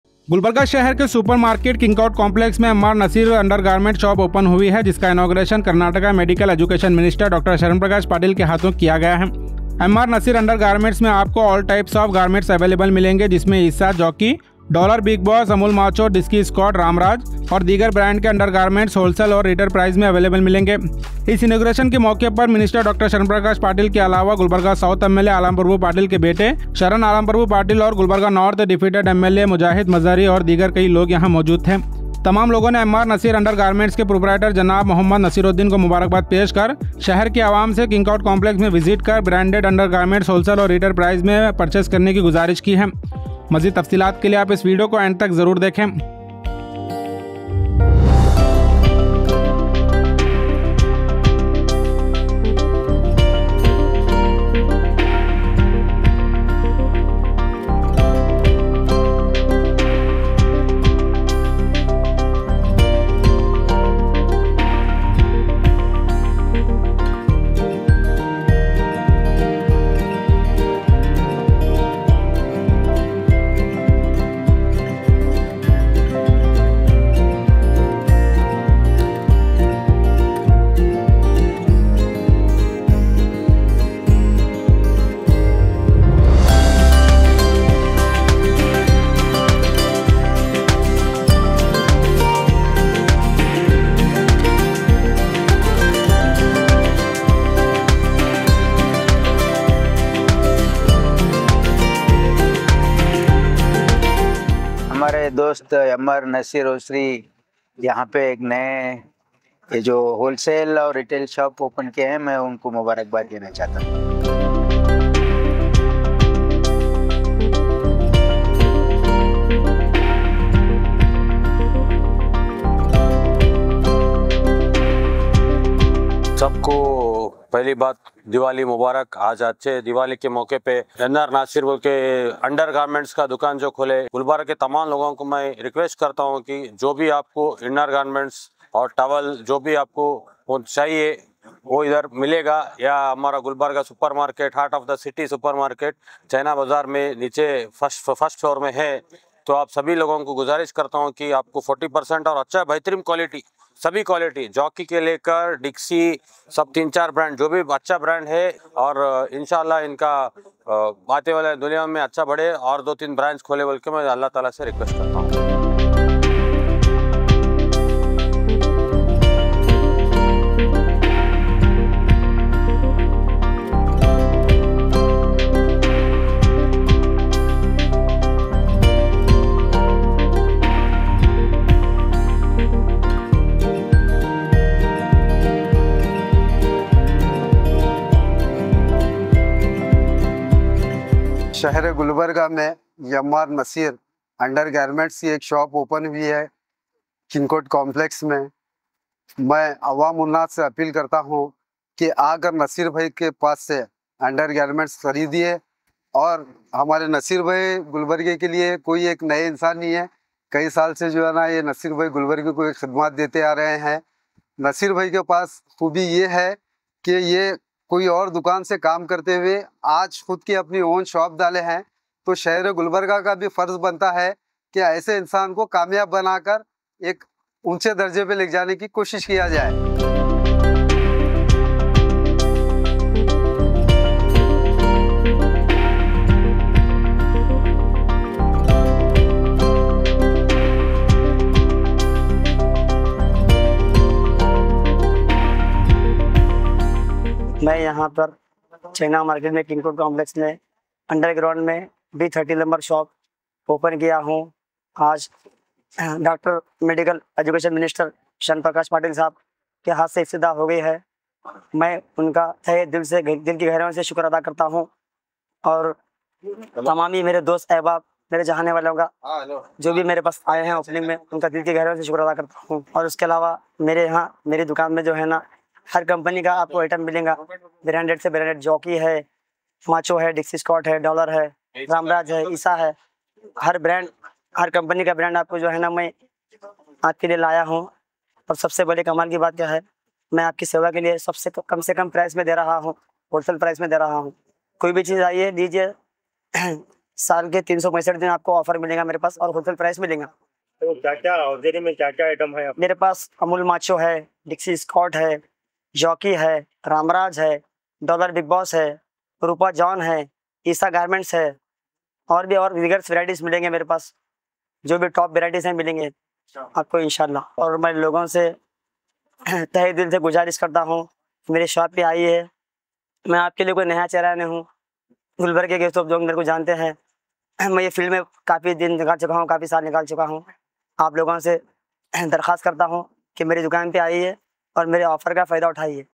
Inauguration of M.R Naseer Under Garment Wholesale & Retail at King Court Complex, Super Market.
Inauguration of M.R Naseer Under Garment Wholesale & Retail located at B-30, King Court Complex, Super Market, Gulbarga.